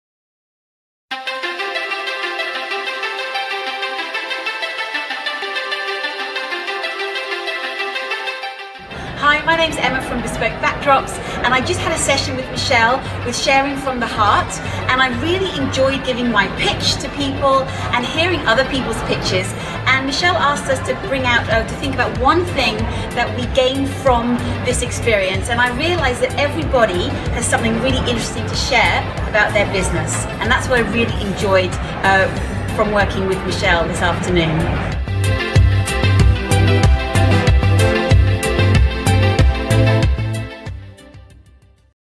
Emma Veiga-Malta: Bespoke Backdrops Testimonial
Testimonial from Emma Veiga-Malta of Bespoke Backdrops after "Sharing the Love" presentation for PSA at the Four Seasons Hotel